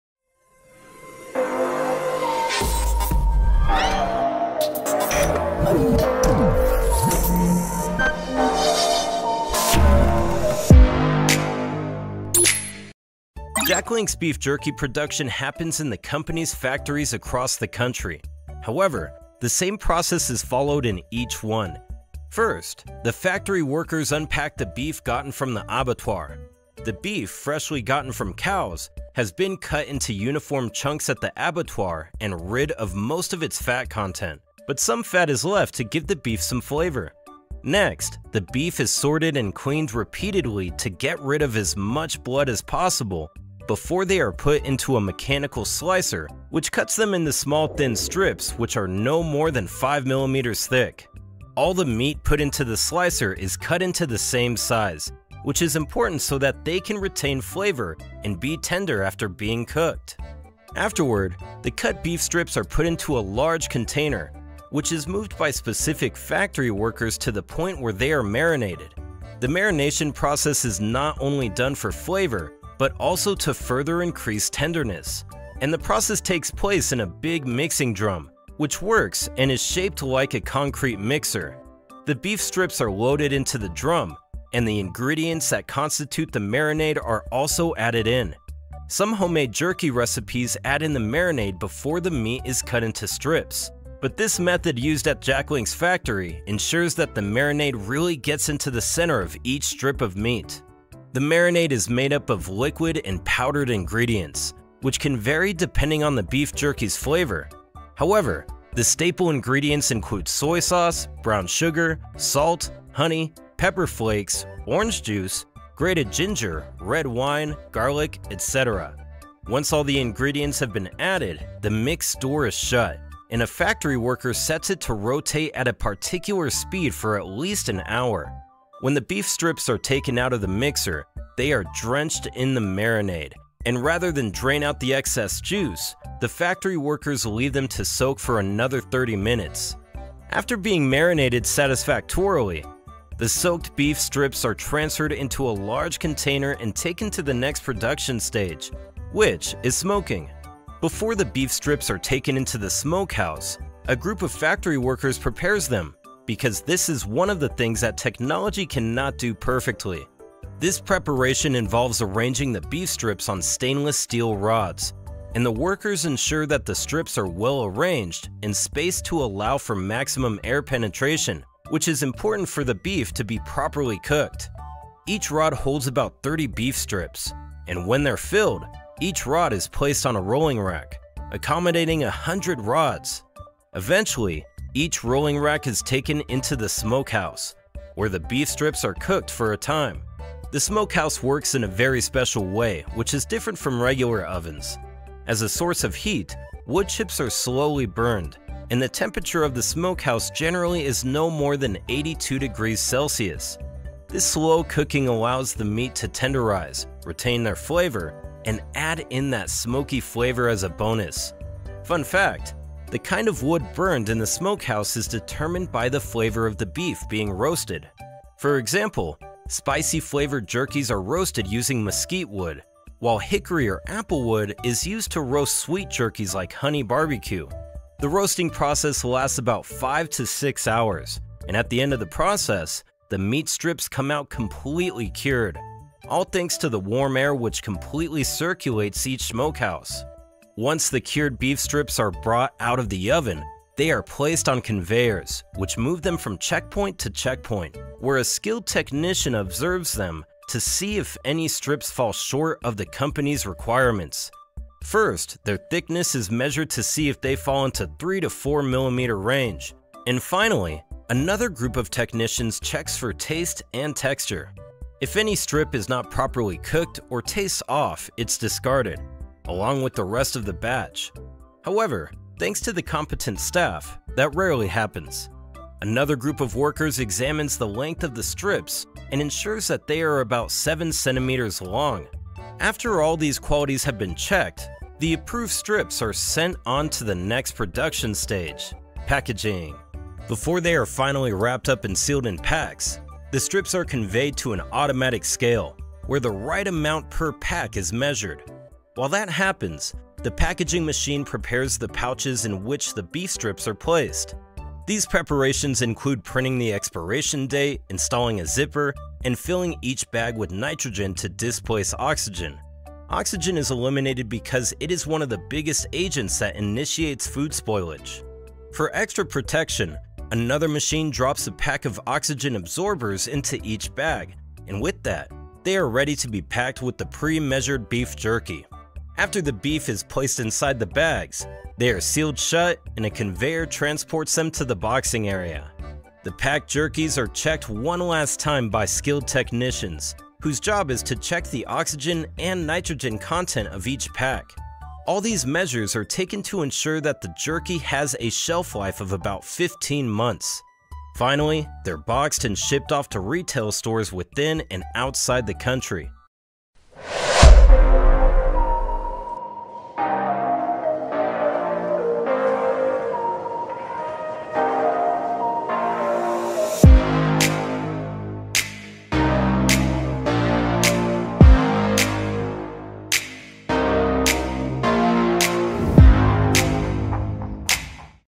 How Jack Link's Beef Jerky Is Made
Are you familiar with dry foods? Many people do not consider dry foods an adequate substitute for fresh foods because, most times, they are so dry they have no moisture content. However, dried foods don’t always come without juiciness or moisture, and Jack Link’s Beef Jerky is a perfect example of that. Although the jerky is literally dried-up beef strips, Jack Link’s protein snacks company manages to make them juicy and tender, unlike many dried proteins, which makes you wonder; how exactly are these snacks made?